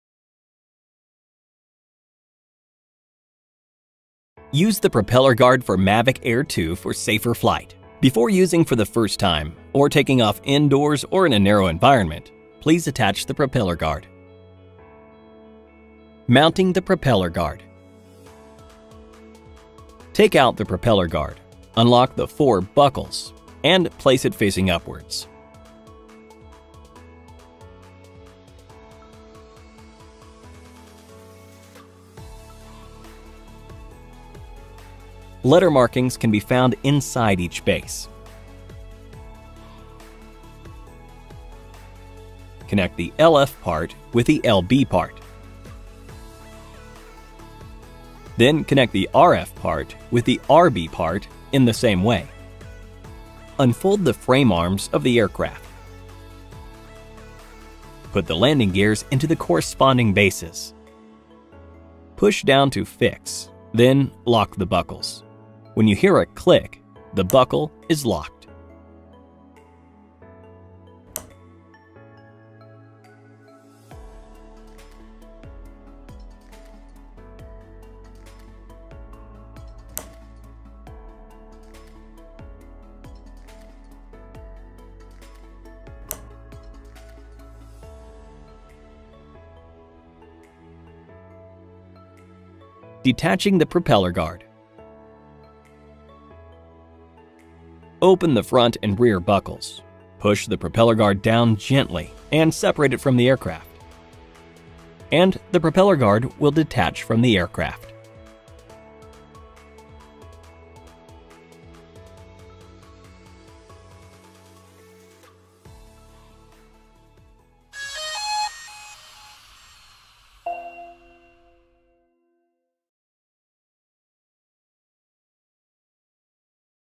Drone Mavic Air 2 - Instala tu protector de hélices
Con solo 570 g, Mavic Air 2 es increíblemente compacto, lo que lo convierte en la herramienta perfecta para llevar su contenido al siguiente nivel. En este video, le mostraremos cómo colocar la protección de la hélice para garantizar un vuelo seguro.

Códigos de tiempo:
0:04 - Introducción
0:17 - Montaje de la protección de la hélice
1:33 - Desmontaje de la protección de la hélice

Visítanos:
Estamos en el Unicenter Shopping de Martinez y en la calle Armenia al 1539 de Palermo